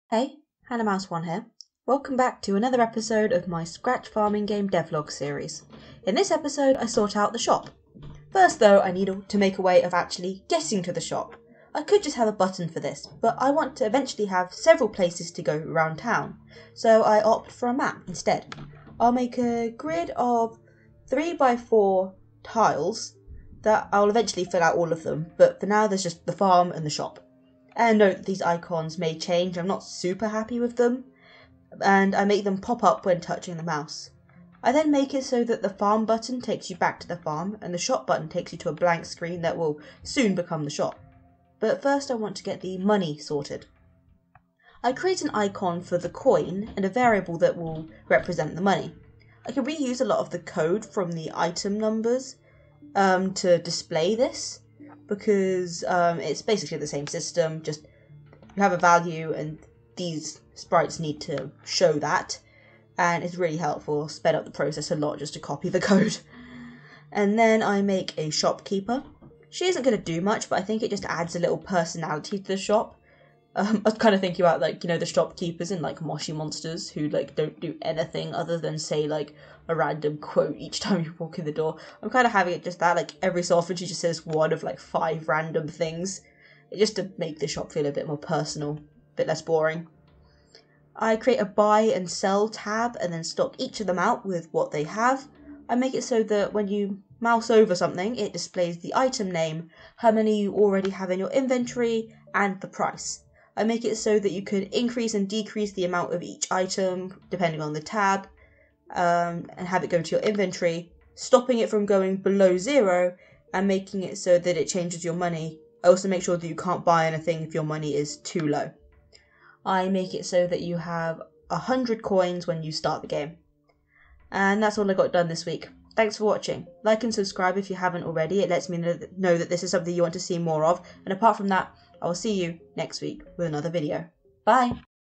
A Farmer's Life - Devlog 8: The Shop
Hey everyone! Welcome to my scratch farming game devlog series! In this episode, I set up the shop!